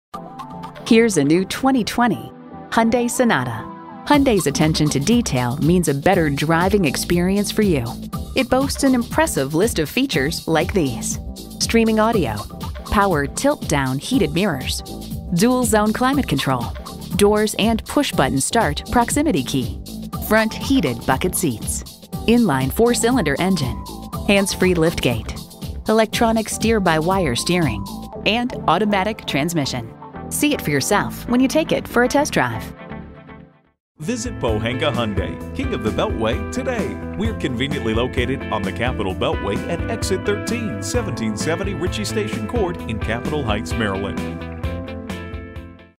New 2020 Hyundai Sonata Capitol Heights MD Washington-DC, MD FLH033801 - SOLD
Year: 2020
Make: Hyundai
Model: Sonata
Trim: SEL
Engine: I4
Transmission: 8-Speed Automatic with Overdrive
Color: Gray
Interior: Black
Stock #: FLH033801
VIN: 5NPEL4JA6LH033801
Gray 2020 Hyundai Sonata 2020 Hyundai Sonata SEL FWD 8-Speed Automatic with Overdrive Black Cloth. I4

Address:
1770 Ritchie Station Court